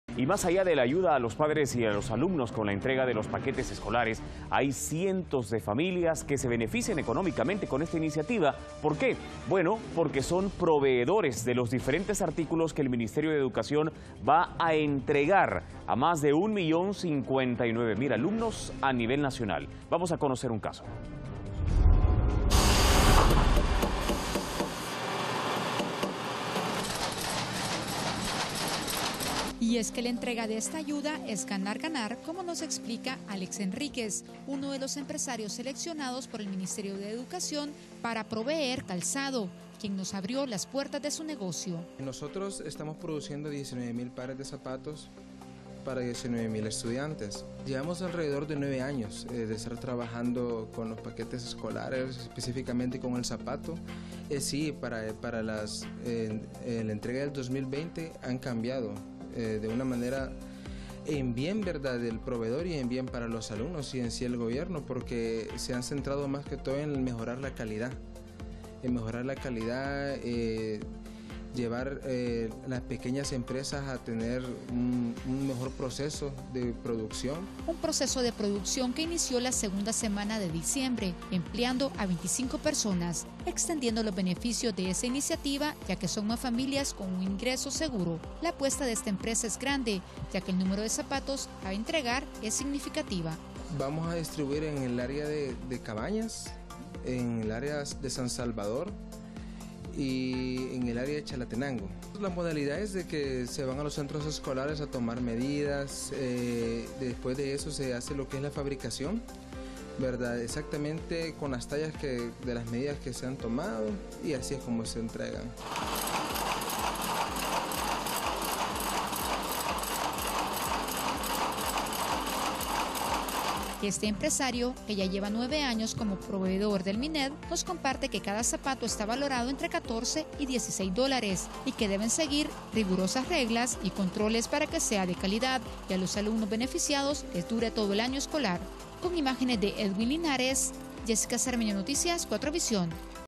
Paquetes escolares 2020
La periodista Jessica Sermeño nos muestra el arduo trabajo que realizan los proveedores seleccionados para elaborar el calzado de los paquetes escolares. Los detalles en la siguiente nota.